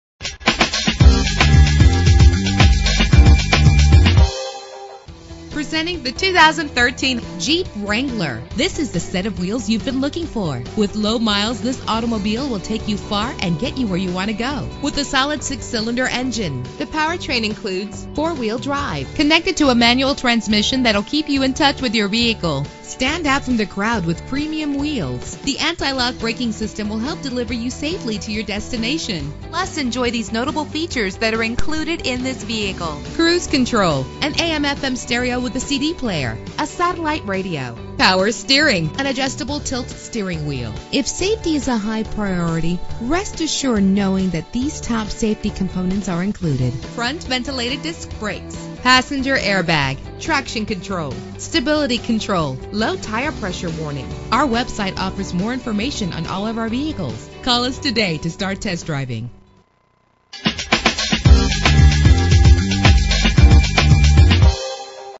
2013 Jeep Wrangler - Lakeland FL
Year: 2013
Make: Jeep
Model: Wrangler
Trim: Sport
Engine: 3.6 liter 6 cylinder 24 valve
Transmission: 6-Speed Manual
Color: Bright White Clearcoat
Mileage: 10429
Address:
5210 S. Florida Ave.